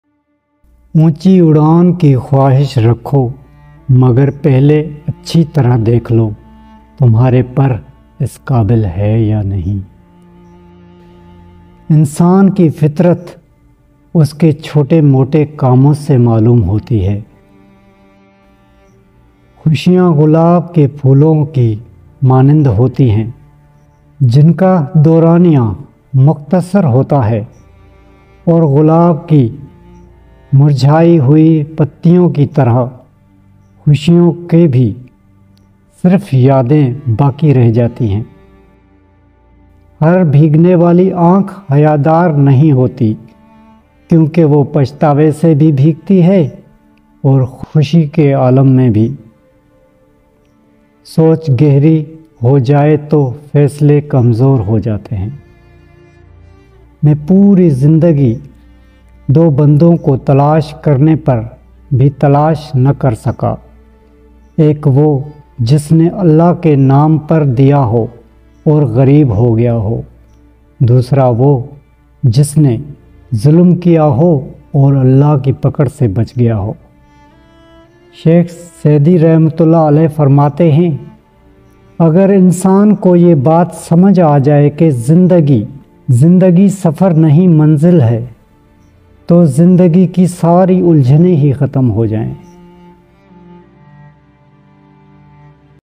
Golden Words of sheikh sadi | Beautiful Quotes | heart touching quotes | Studio one
Artist Voice: M. Hashim

Editor: Tariq Amar

Selection: Tariq Amar

Label: Studio one

Production: Studio one

Uploaded By: Studio one (YouTube Channel)

sheikh sadi
sheikh saadi quotes
sheikh saadi ke aqwal